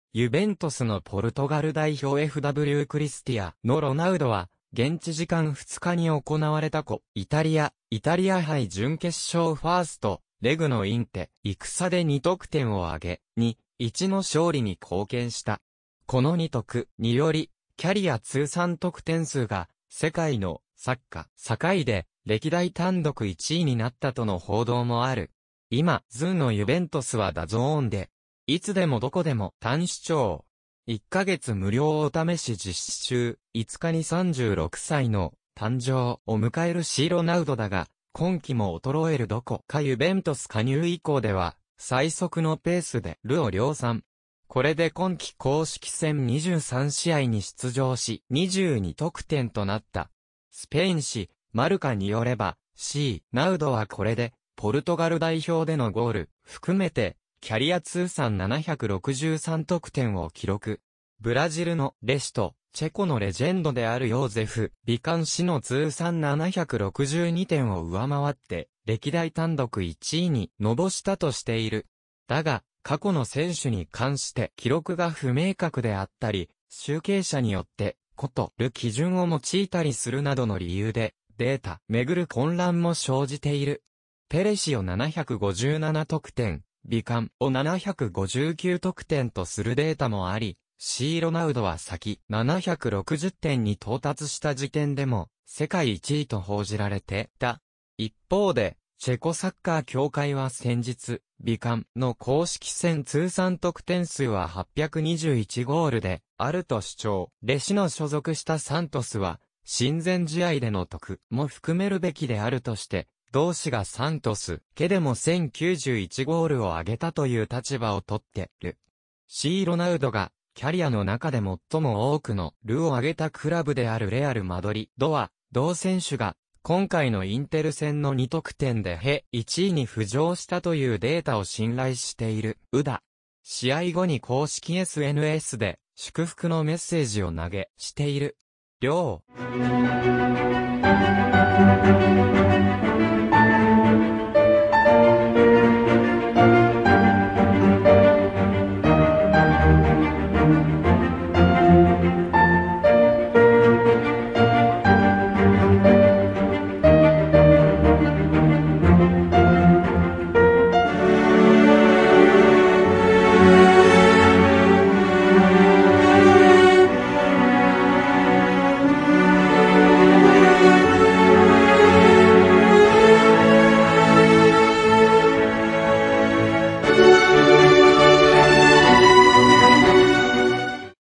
C・ロナウド、今度こそ通算ゴール数世界歴代1位？763得点でペレ氏超えか
ユベントスのポルトガル代表FWクリスティアーノ・ロナウドは現地時間2日に行われたコッパ・イタリア（イタリア杯）準決勝1stレグのインテル戦で2得点を挙げ、2-1の勝利に貢献した。この2得点により、キャリア通算得点数が世界のサッカー界で歴代単独1位になったとの報道もある。 【今シーズンのユベントスはDAZNで！ いつでもどこでも簡単視聴。1ヶ月無料お試し実施中】 　5日に36歳の誕生日を迎えるC・ロナウドだが、今季も衰えるどころかユベントス加入以降では最速のペースでゴールを量産。これで今季公式戦23試合に出場して22得点となった。 　スペイン紙『マルカ』によれば、C・ロナウドはこれでポルトガル代表でのゴールも含めてキャリア通算763得点を記録。ブラジルのペレ氏と、チェコのレジェンドであるヨーゼフ・ビカン氏の通算762点を上回って歴代単独1位に浮上したとしている。 　だが過去の選手に関しては記録が不明確であったり、集計者によって異なる基準を用いたりするなどの理由で、データを巡る混乱も生じている。ペレ氏を757得点、ビカン氏を759得点とするデータもあり、C・ロナウドは先月760点に到達した時点でも世界1位と報じられていた。 　一方でチェコサッカー協会は先日、ビカン氏の公式戦通算得点数は821ゴールであると主張。ペレ氏の所属したサントスは親善試合での得点も含めるべきであるとして、同氏がサントスだけでも1091ゴールを挙げたという立場を取っている。 　C・ロナウドがキャリアの中で最も多くのゴールを挙げたクラブであるレアル・マドリードは、同選手が今回のインテル戦の2得点で歴代1位に浮上したというデータを信頼しているようだ。試合後に公式SNSで祝福のメッセージを投稿している。 【了】